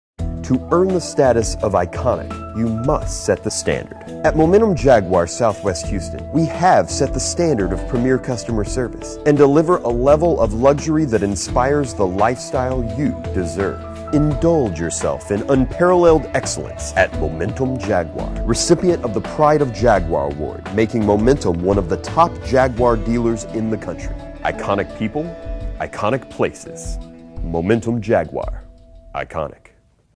Momentum Jaguar - Iconic - Private Jet at Sugarland Airport
Momentum Jaguar is Houston's largest Jaguar dealer and recently given the Pride of Jaguar Award making us one of the top Jaguar dealers in the country.

Jaguar has been an Icon representing the ultimate luxury, prestige, performance and style. Take a peek into the lives of some well known Houstonians living the Iconic life with their Jaguar.